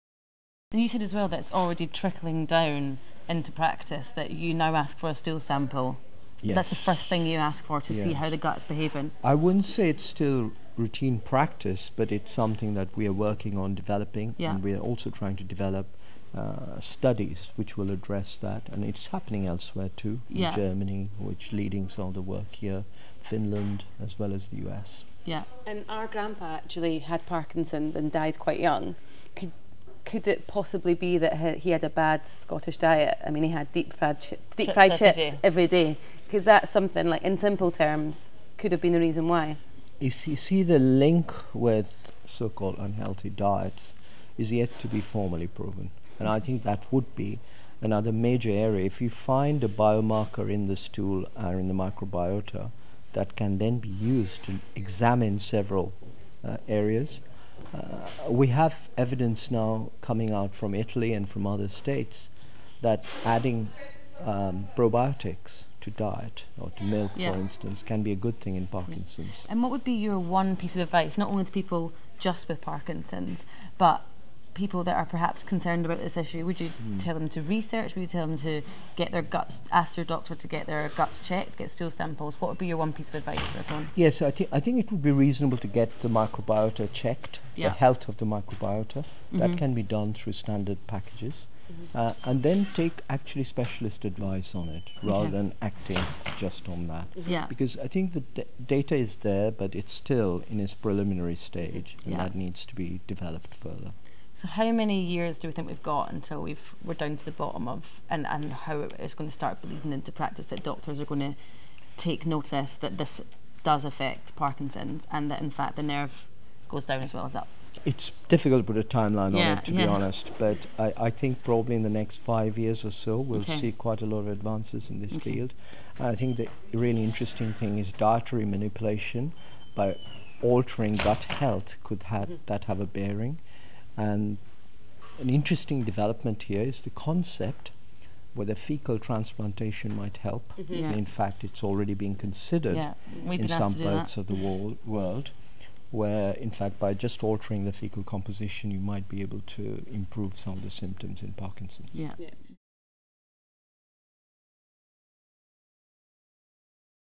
Will the Way Parkinson's Is Treated Change?
Answered in this video:

Is your new discoveries with links to Gut Health trickling down in practice?
Is there a link with unhealthy diet and Parkinson's?
What is your one piece of advice for people with Parkinson's and gut health?
How many years of research are we away from getting definite answers?

Ray Chaudhuri, Professor of Movement Disorders and Director Parkinson's International Centre of Excellence, King’s College London

For more information such as recipes, further reading, articles and advice, head over to